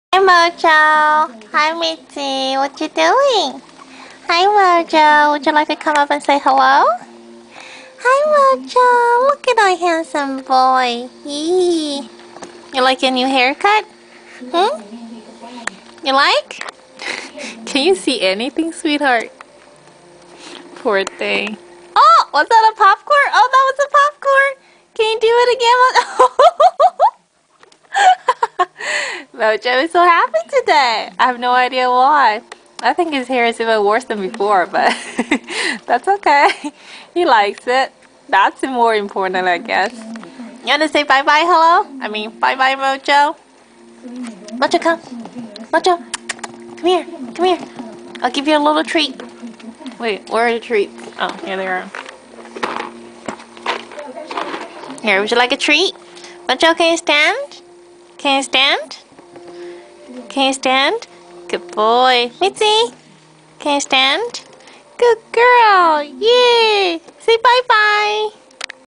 Poppy Mojo!
Wanted to take a video of Mojo's new haircut, then he started to get all excited and ta-da, popcorn extravaganza.